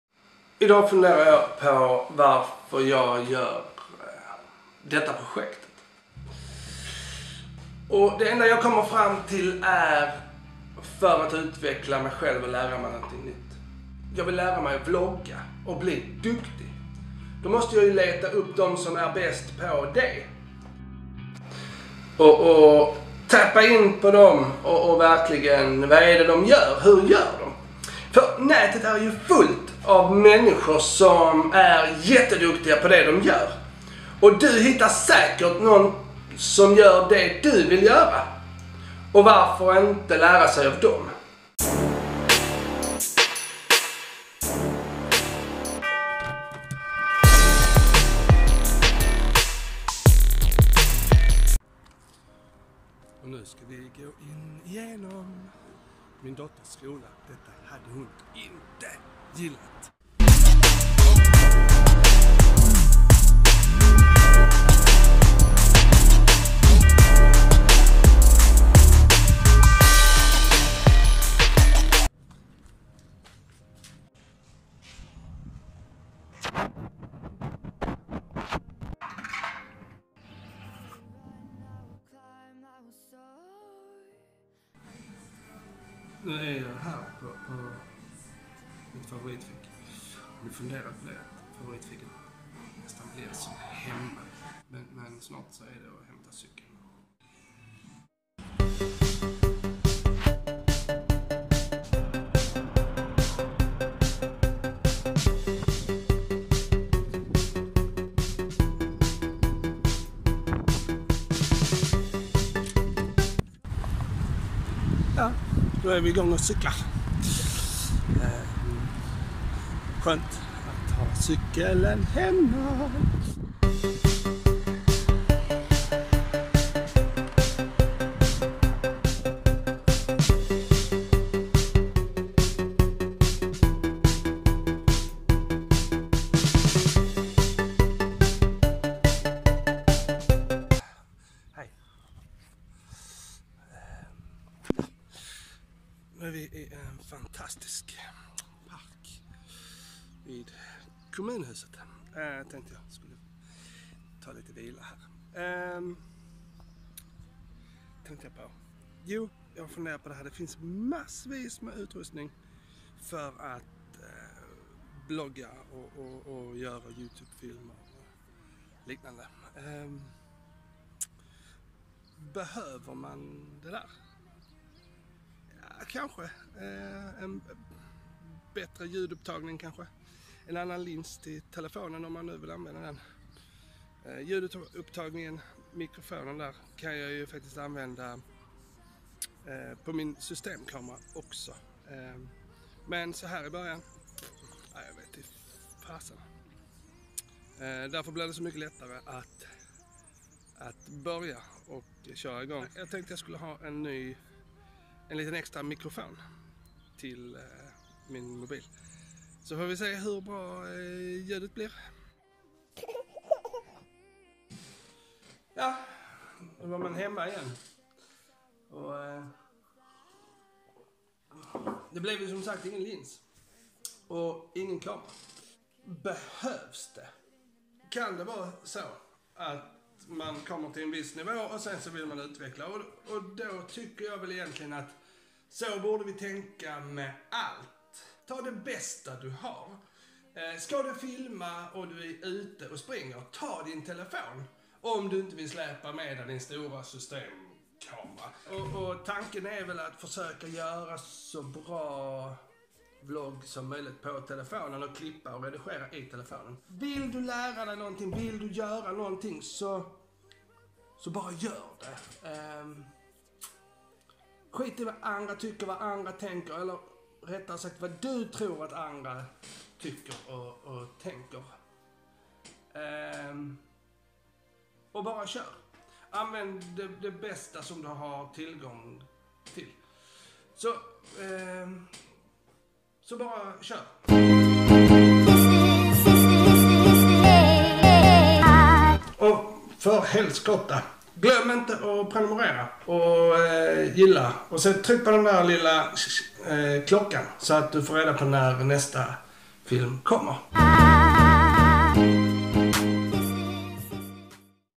Hur F%&N ska detta gå?
Hur F%&N ska detta gå? En reflektion över rädslan för att prova